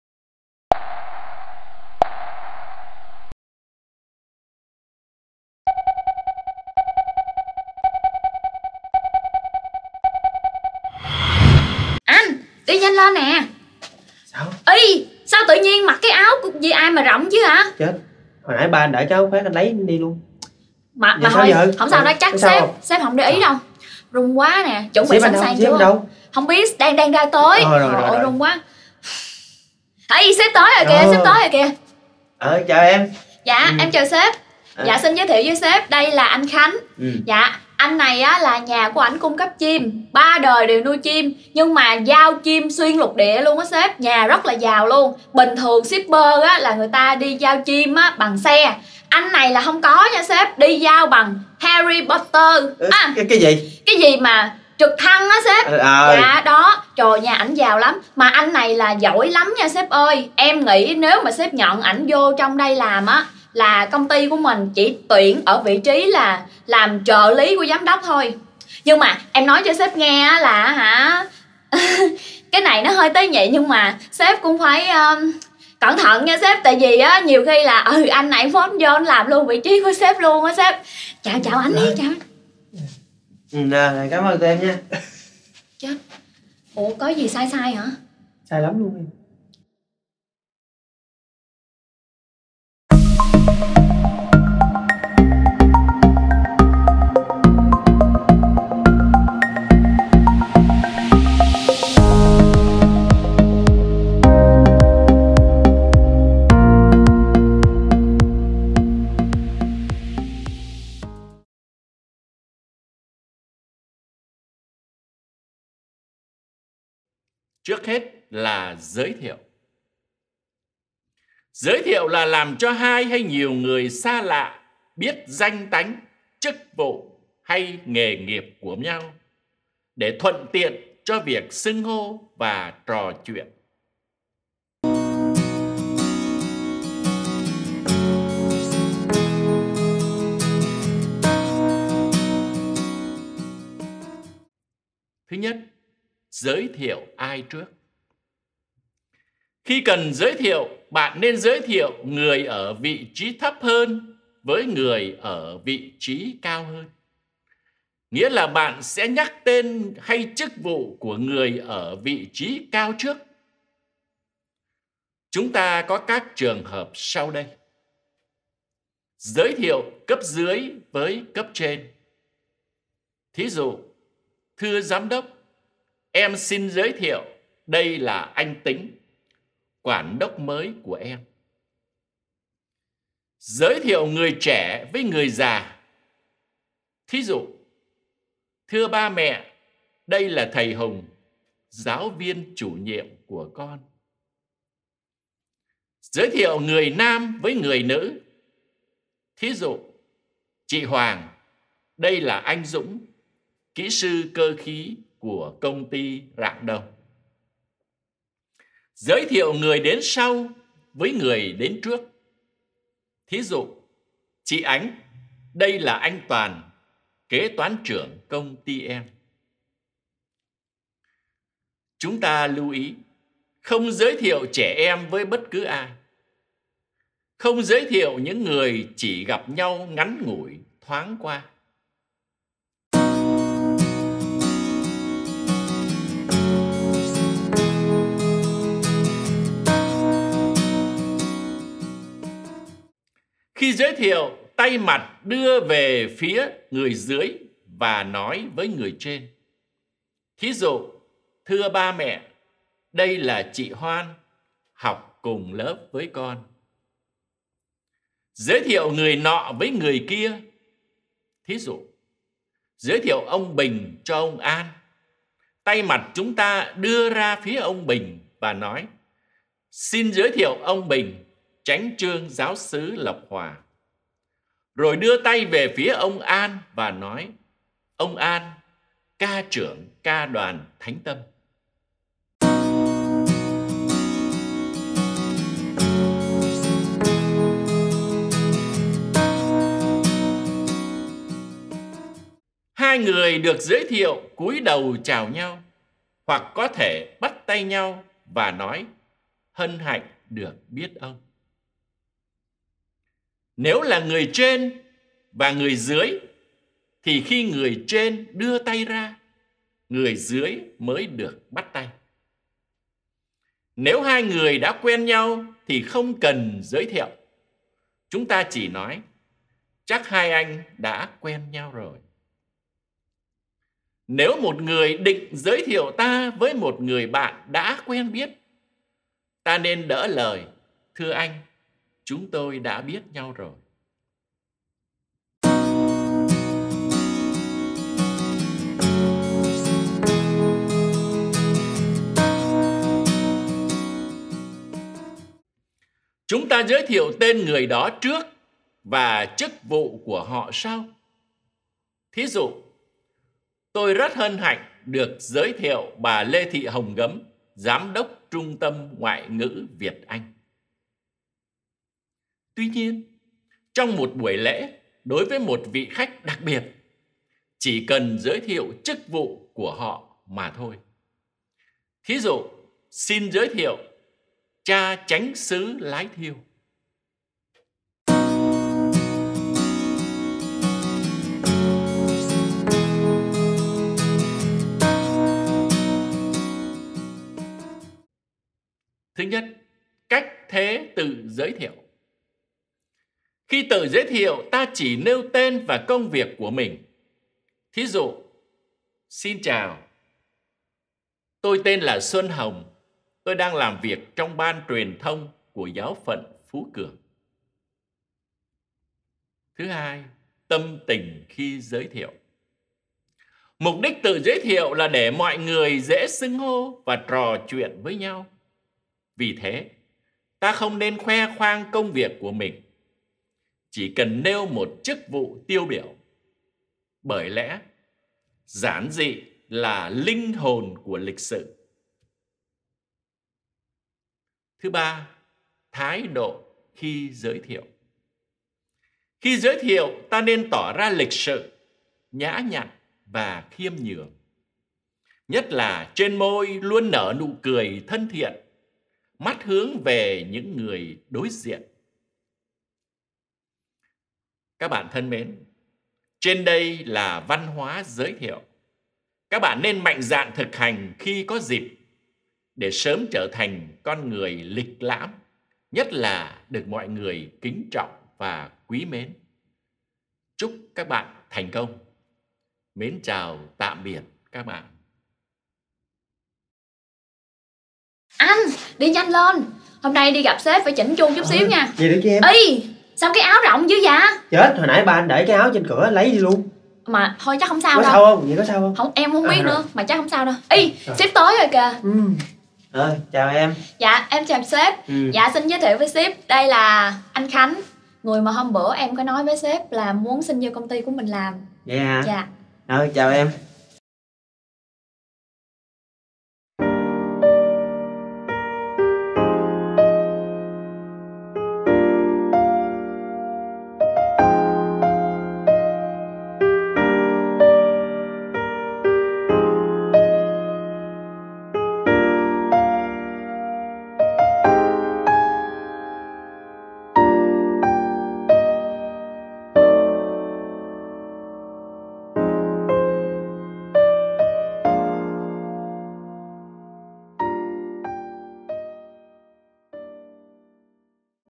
Văn hoá giao tiếp: Bài 4 - Văn hóa giới thiệu
Các nghi thức trong phép xã giao khá đơn giản, nhưng không phải ai cũng hiểu biết và nhận thức được tầm quan trọng của nó. Nếu biết áp dụng các quy tắc giới thiệu thì bạn sẽ chiếm được cảm tình của mọi người, và để lại ấn tượng sâu sắc trong lòng người đối diện.